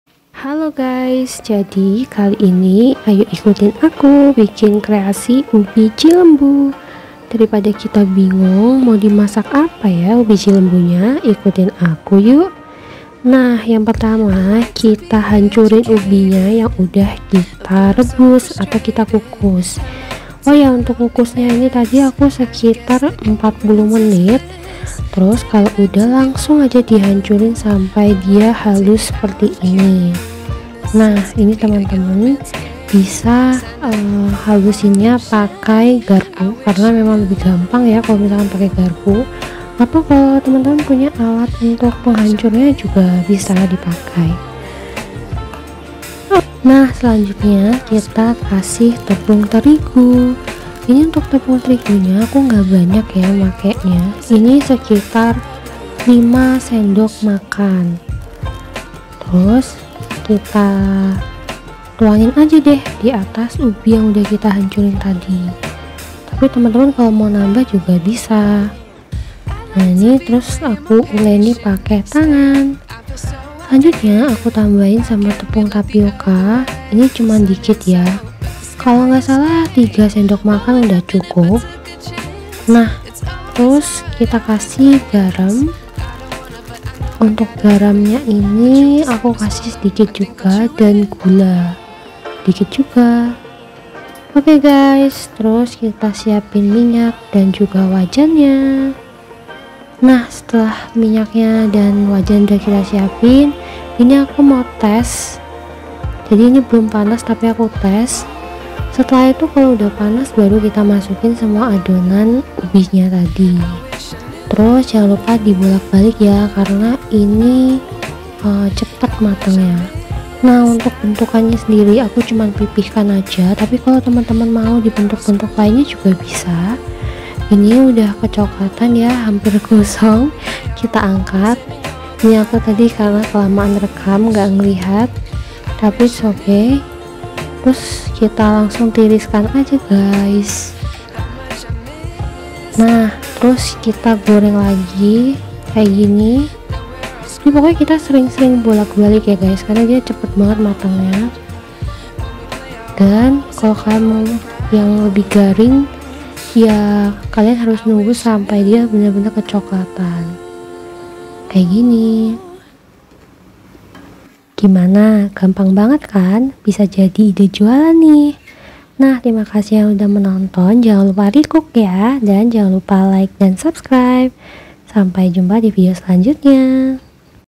RESEP CAMILAN SIMPLE & HEMAT UTK MAHASISWA
Resep Ubi Goreng Madu:

Bahan:
- Ubi 100 gram (bisa diganti ubi apa saja)
-  10 Sdm (sendok makan) tepung terigu
-  5 sdm (sendok makan) tepung tapioka
- secukupnya gula dan garam
- air

yuk bikin kreasi jajanan yang mudah di kost!